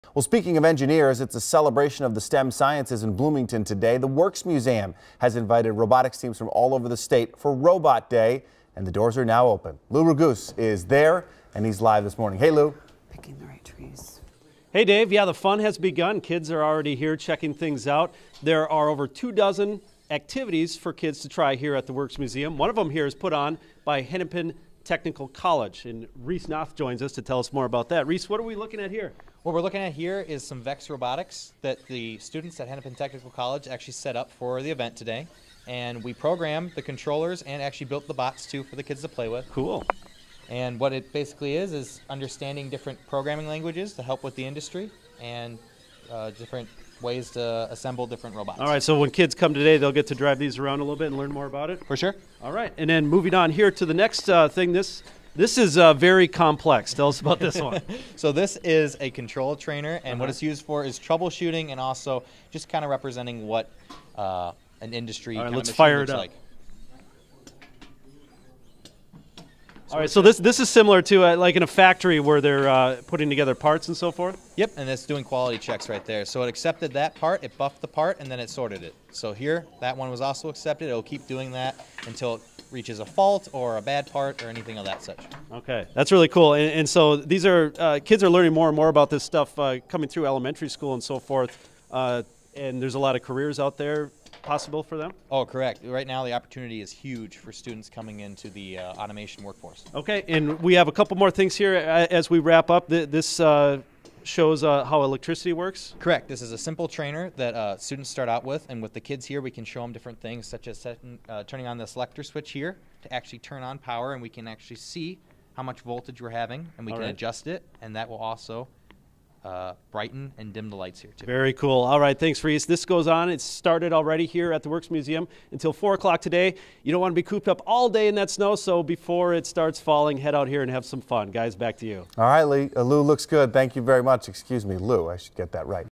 Robot Day at The Works Museum
Lou Raguse visits with Hennepin Technical College at Robot Day to talk about the exhibits they have for students to try out.